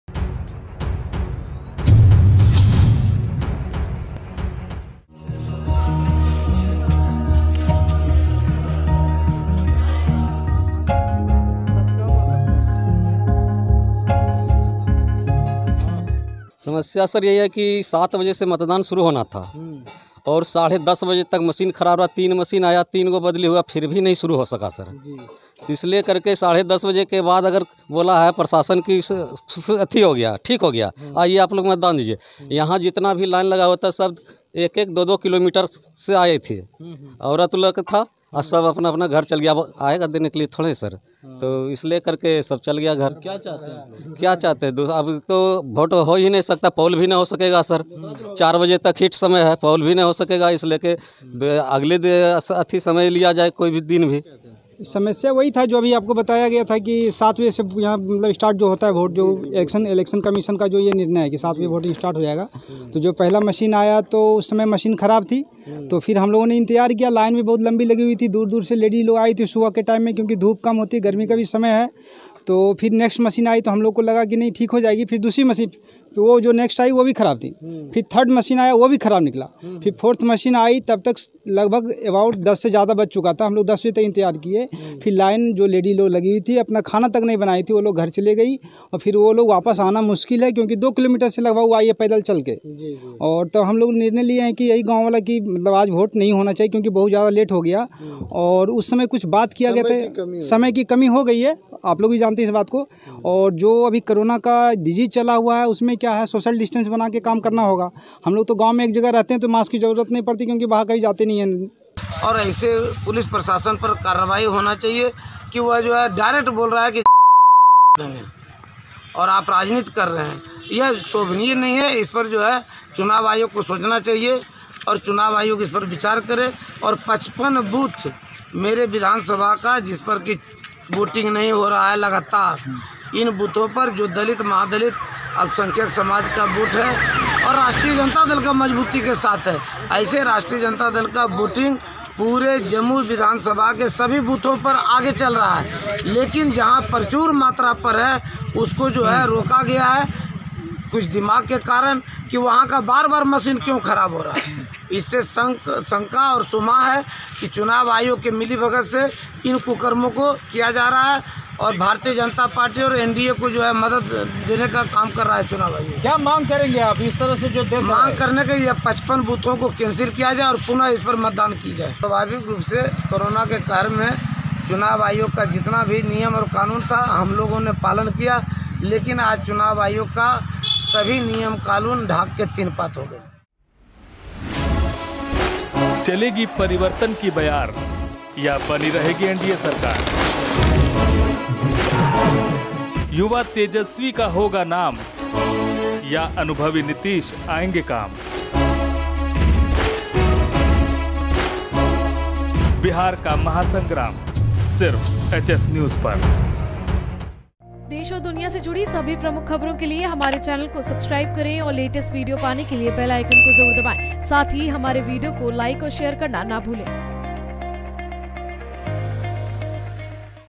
Jamui Vidhan Sabha के सतगामा गांव में EVM खराब, ग्रामीणों ने मतदान से किया इंकार | Bihar Election
जमुई विधानसभा 241 के सतगामा गांव में लगातार चौथी बार EVM खराब हुई...